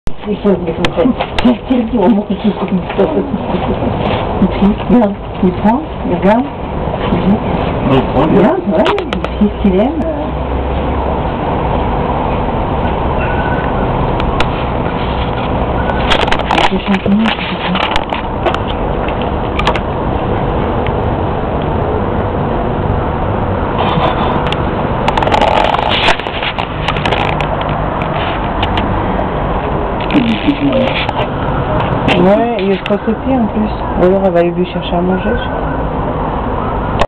Squirrel monkeys at Doué zoo in France
Video of a baby squirrel monkey and it's mother at Doué zoo in France, recorded on a visit there in September 2008..